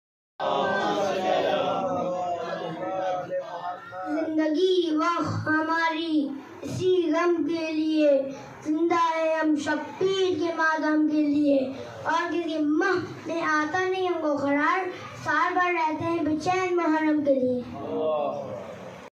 Peshkhhani by Syed Akbar Raza|2 muharram 1442 H
Aza-E-Nawada
|Darbare Hussaini|
Nawada Kalan, Hajipur ¬– Vaishali, Bihar
Labbaik Ya Hussain (as)
Labbaik Ya Abbas (as)

Muharram 2020